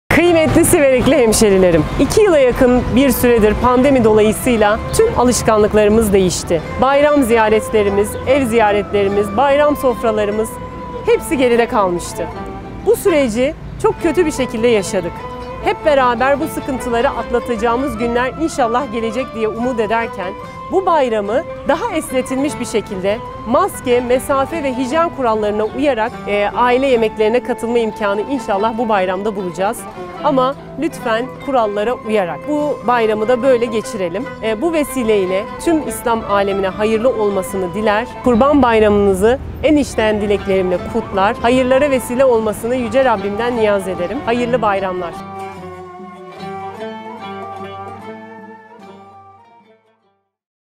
Belediye Başkanımız Ayşe Çakmak'tan Kurban Bayramı mesajı
Siverek Belediye Başkanı Ayşe Çakmak, Kurban Bayramı dolayısıyla yayımladığı mesajda pandemiyle geçen son bayram olması temennisinde bulundu.